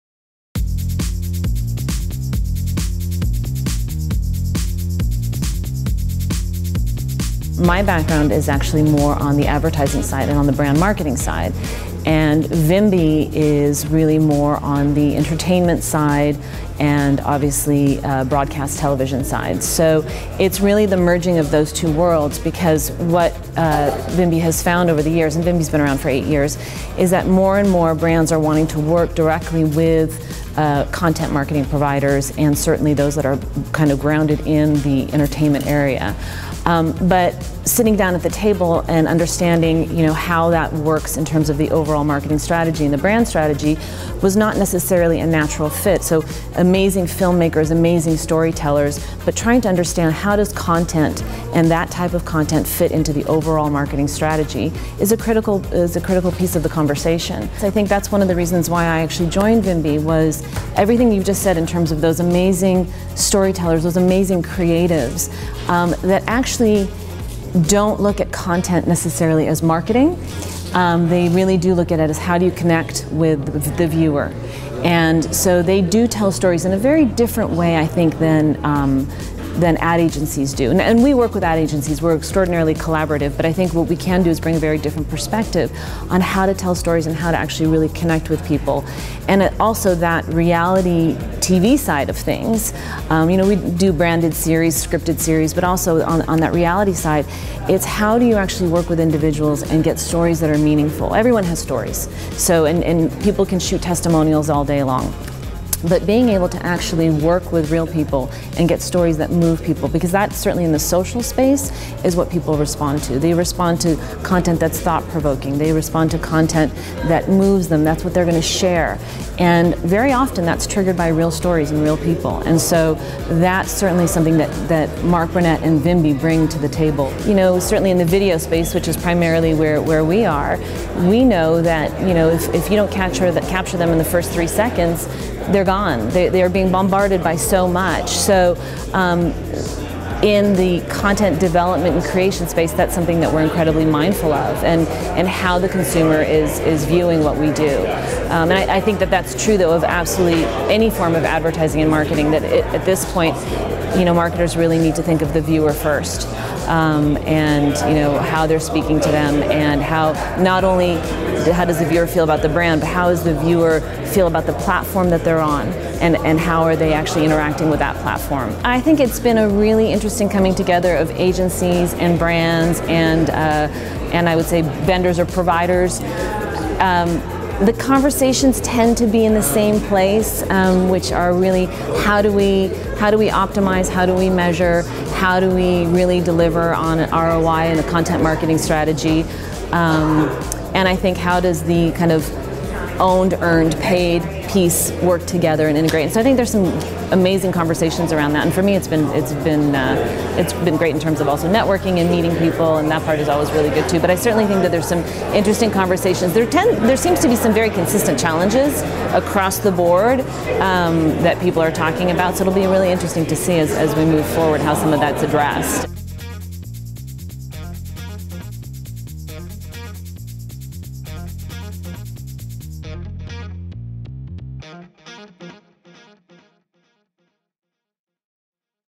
Create Content that Creates Connections - Liza Pegram
As the SVP Strategy and Development at VIMBY, The Mark Burnett Content Studio, Liza Pegram understands the need to merge the world of advertising and brand marketing with that of entertainment and broadcast TV. For 8 years, VIMBY has been helping brands understand how storytelling and creative content can fit into their overall marketing strategy and help them better engage their audience. In this interview from the ADAPT summit, Liza shares how marketers can put the viewer first to make real connections.

Featuring:
Liza Pegram, SVP Strategy and Development at VIMBY, The Mark Burnett Content Studio